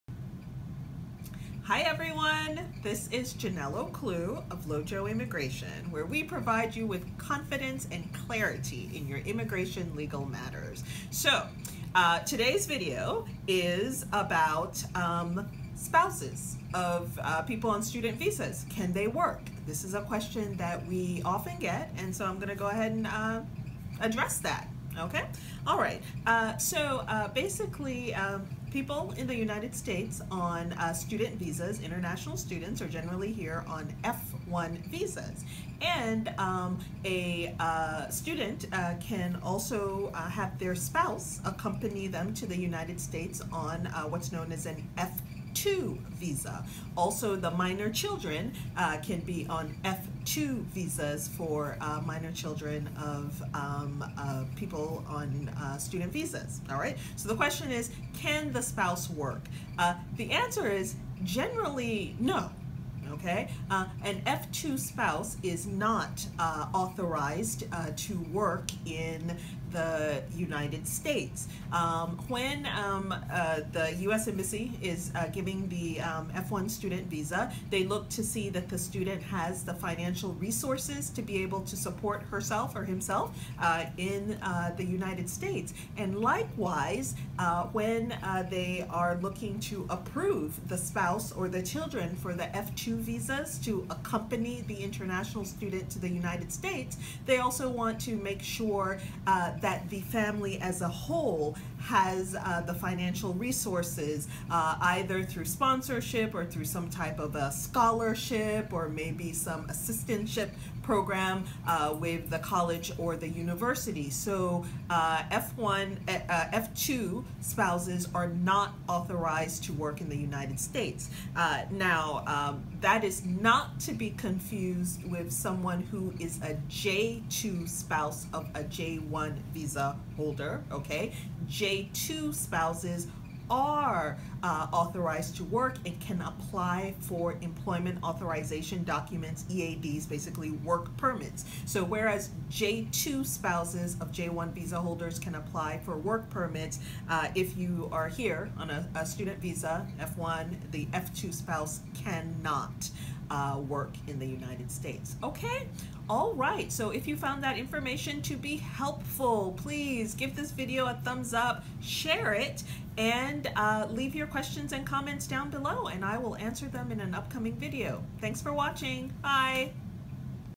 Are spouses of F1 Students allowed to work in the US?
Attorney Ocloo describes the F2 visa available for spouses of international students and whether they can receive employment authorization.